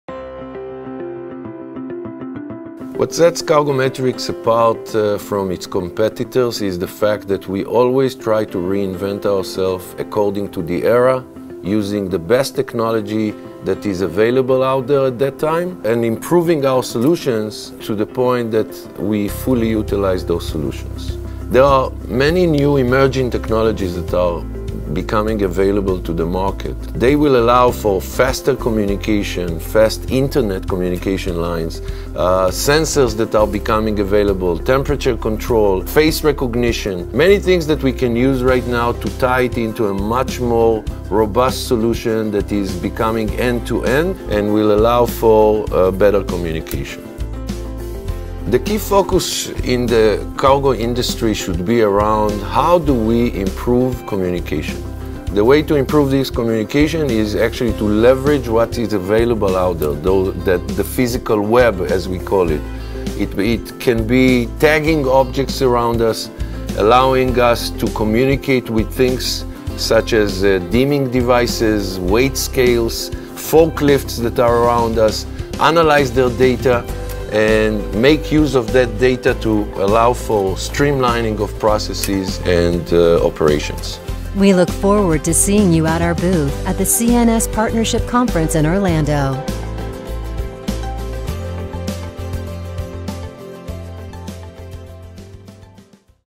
CargoMatrix Inc.
CargoMatrix CEO Eytan Sheerit interview April 2017